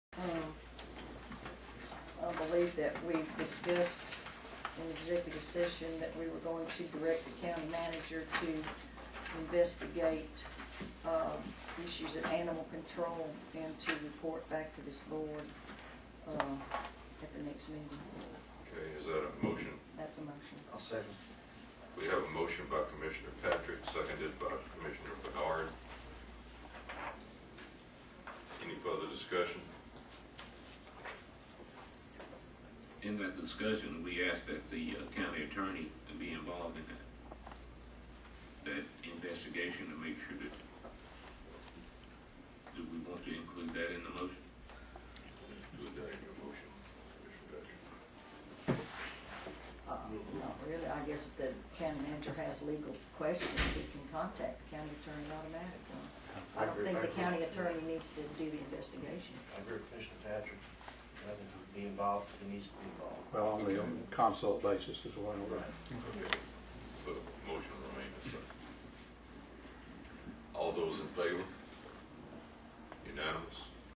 Investigation of animal control facility
The BOC directs the County manager to investigate conditions at the animal control facility.  This raises a question as why the County manager to  investigation something he already has responsibility for.  It's like putting the fox in charge of the henhouse.